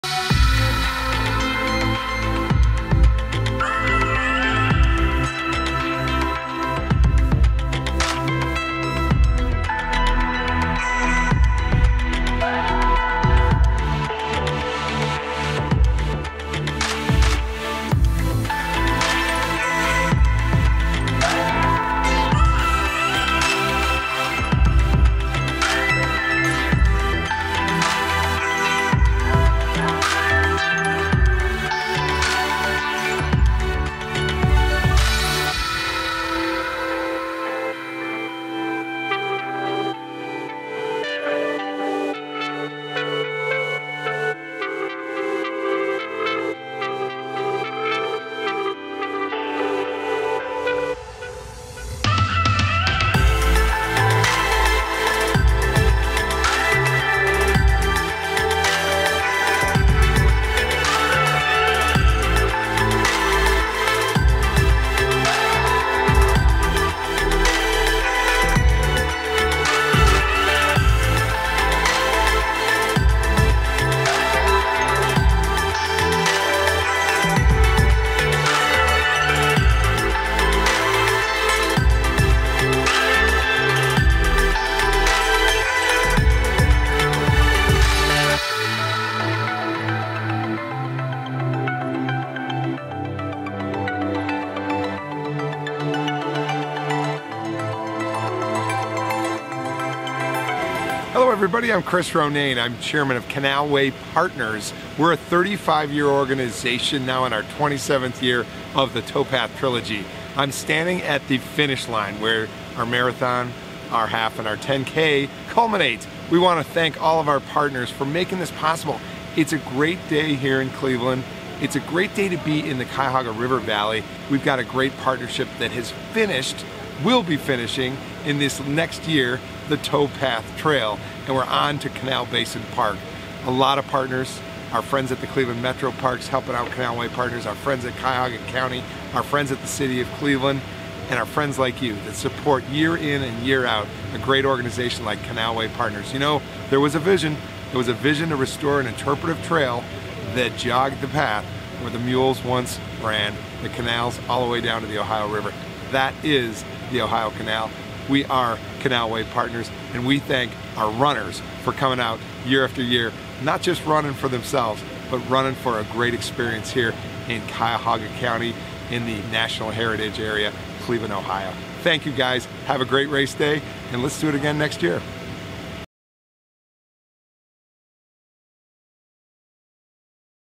2020 Towpath Marathon
A few of the sights during Canalway Partners' Towpath Marathon, Half Marathon, and 10k on October 11, 2020. The third leg of the Towpath Trilogy was held inside Cleveland Metroparks Ohio & Erie Canalway Reservation.

Events like the Towpath Marathon directly support the development of the Towpath Trail, Canal Basin Park, and dozens of projects within the Ohio & Erie Canalway National Heritage Area, projects that make our community stronger and more vibrant.